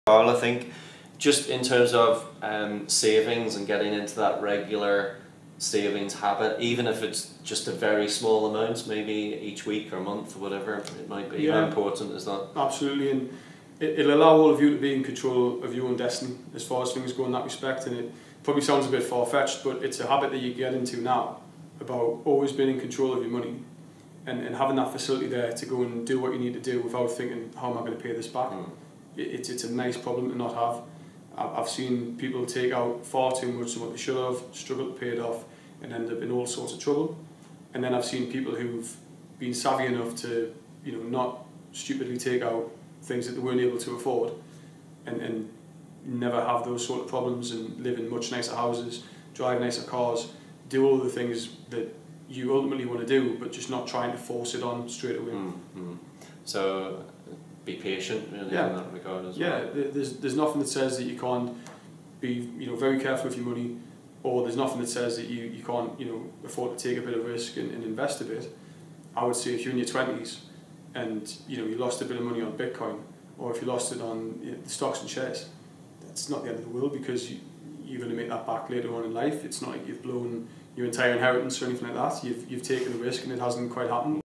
Develop a regular savings habit
Virgin Money's Structural Liquidity Manager, Mr Karl Morgan, discusses the importance of developing a regular savings habit.